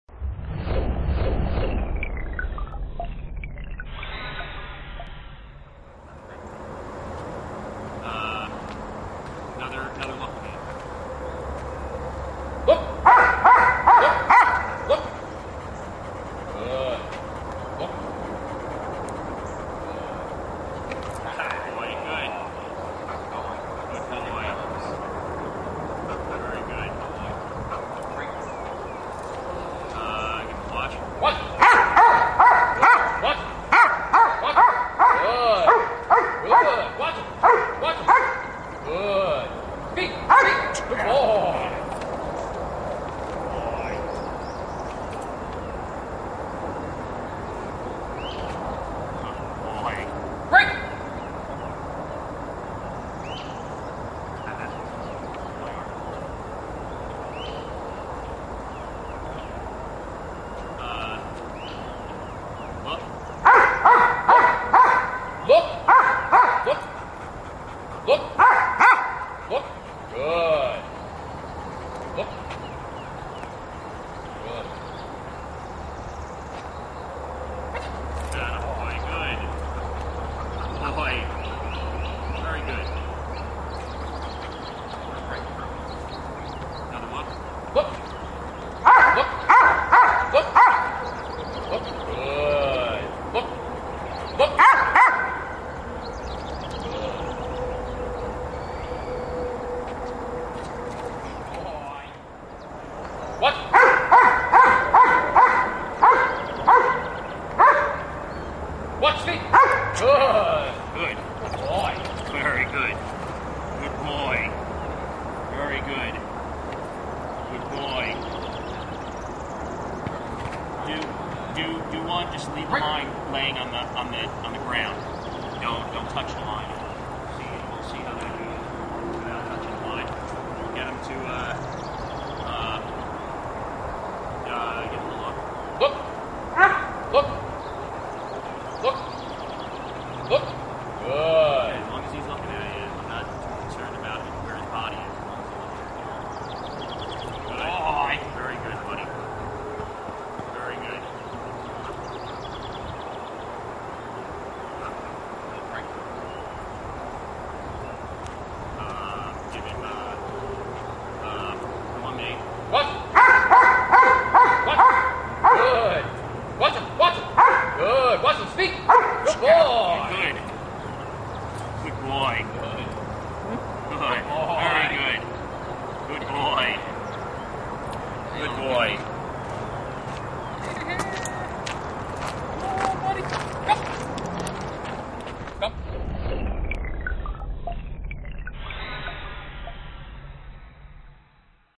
Teaching Look vs Watch Exercise
Here we are using a remote collar to help teach the look vs watch exercise with a police service dog. It's just another exercise to build attention and focus as well as multitasking by remaining on a place table, being able to direct the dog attention where you want it, work the dogs grip and play ball.